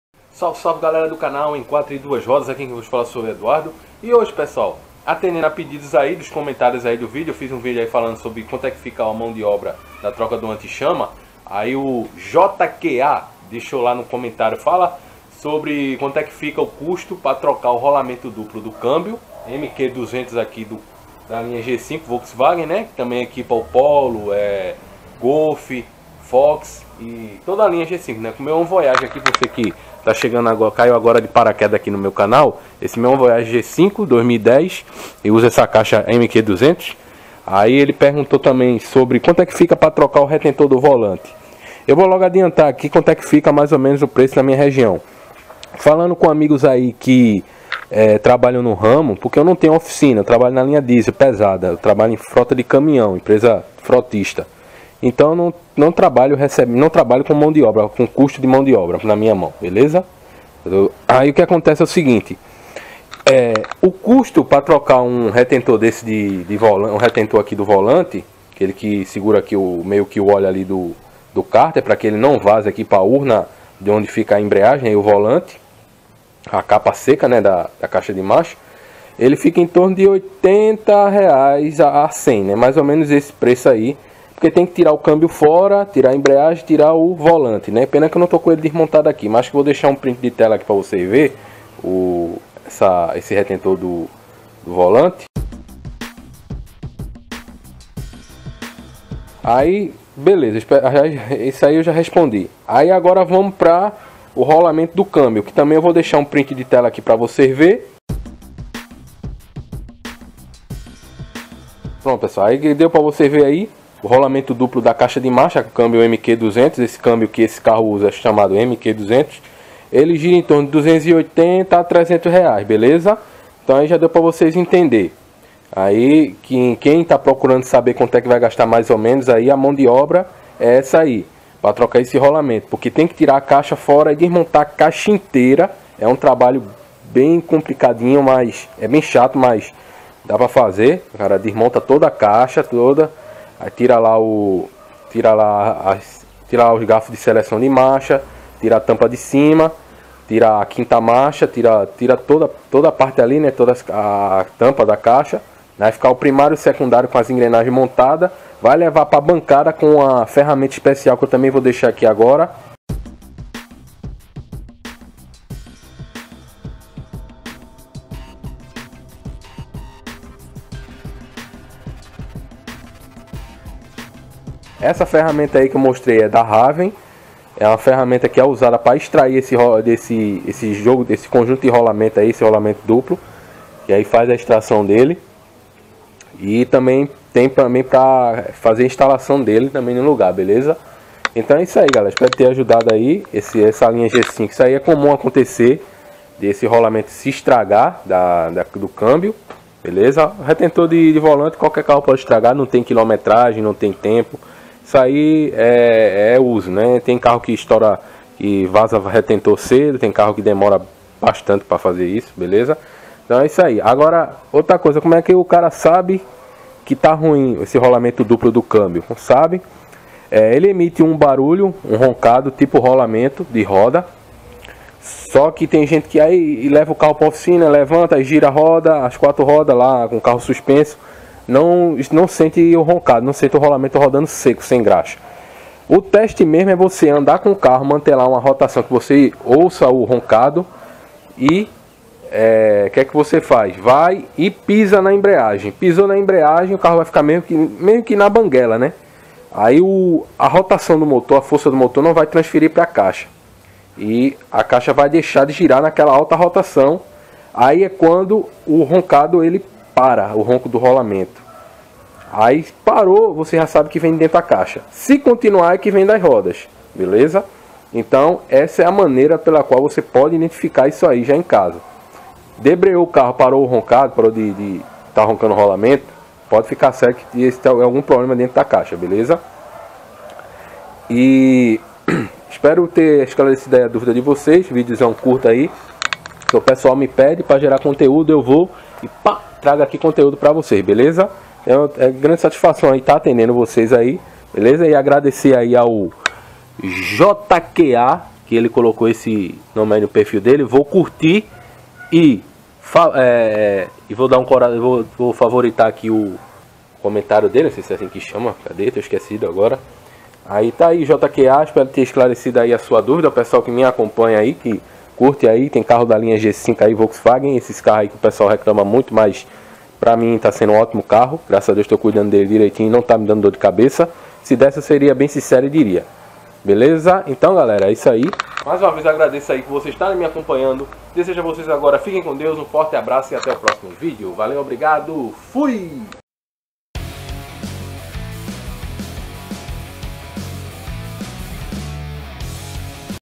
PREÇO PARA TROCA DO ROLAMENTO DUPLO DA CAIXA MQ200 GOL G5 E DERIVADOS !
SALVE AMIGOS MAIS UMVÍDEO SOBRE A TROCA DO ROLAMENTO DUPLO DA CAIXA MQ200 QUE EQUIPA A LINHA VOLKSWAGEN !